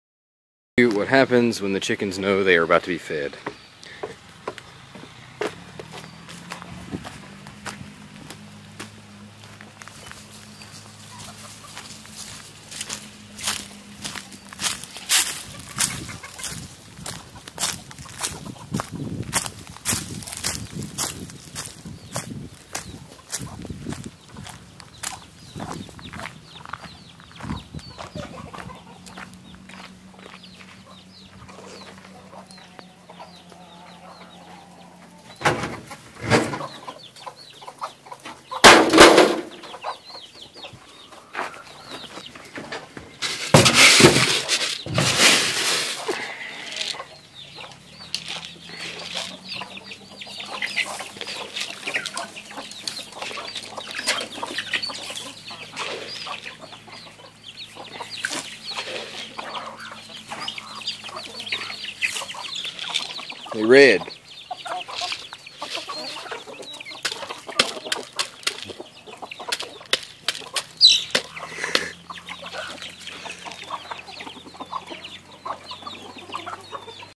Feeding the Chickens
Just a typical afternoon feeding our flock. they're a bit spoiled.